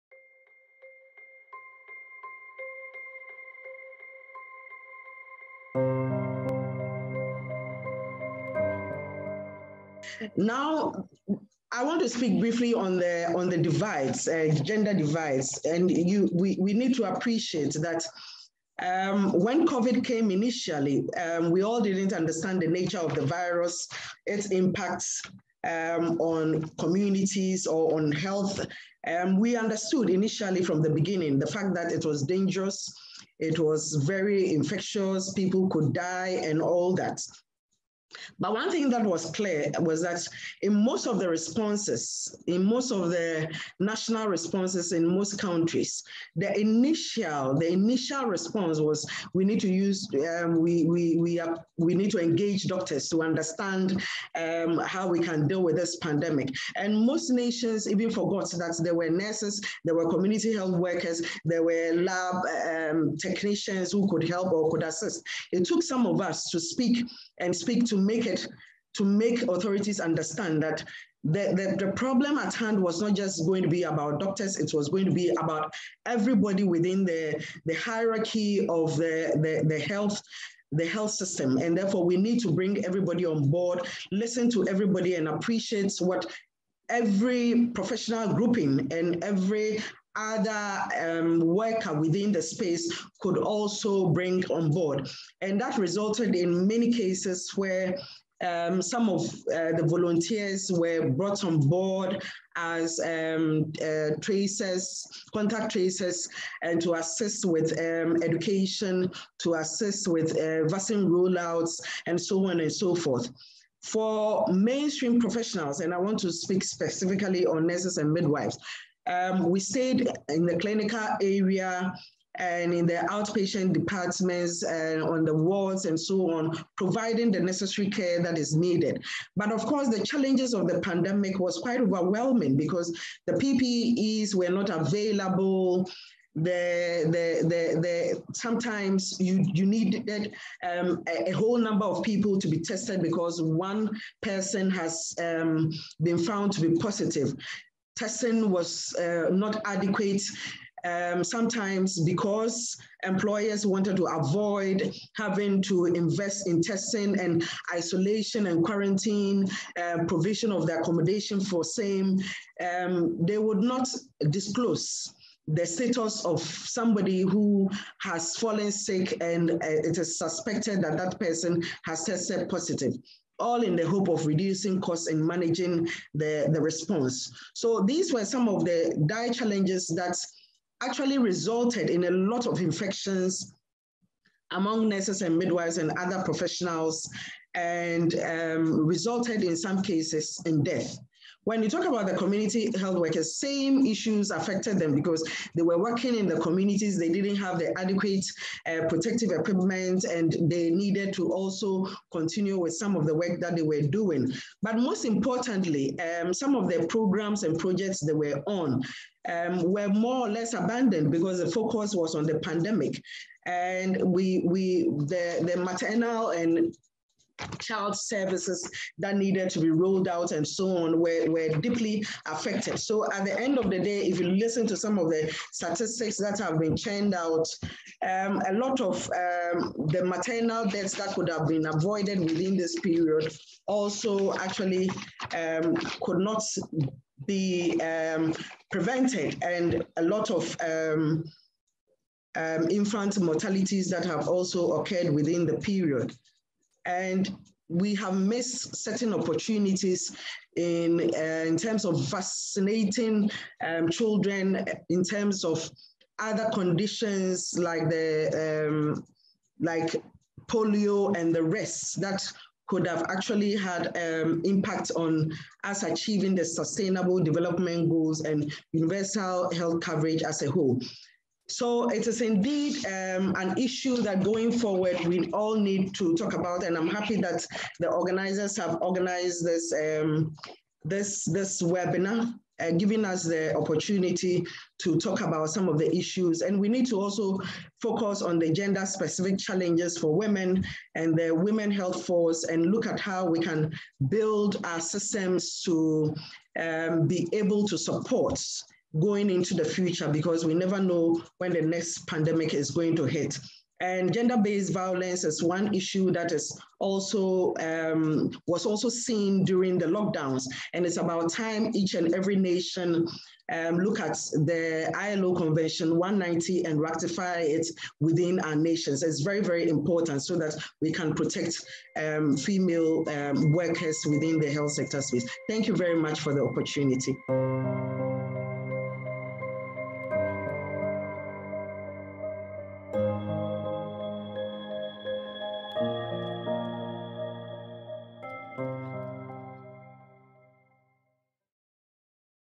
How Covid 19 deepened gender divides in health systems and society
Perpetual Ofori—Ampofo

The President of Ghana Registered Nurses
And Midwives association and Chairperson
Of the West African Health Sector| Unions Network (WAHSUN)Of Public Services International (Ghana )